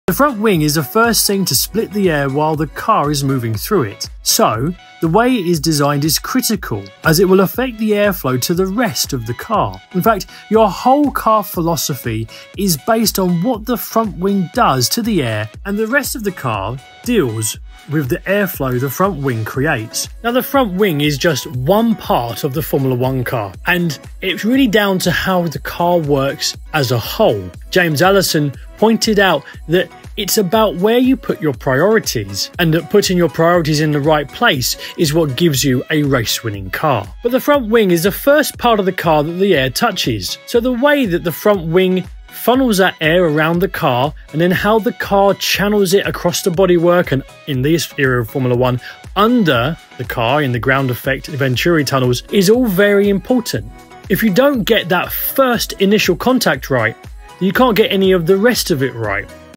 How The Front Wing Works…
Every part of the F1 Car is important but the front wing is where everything starts, let’s look at how the front wing works.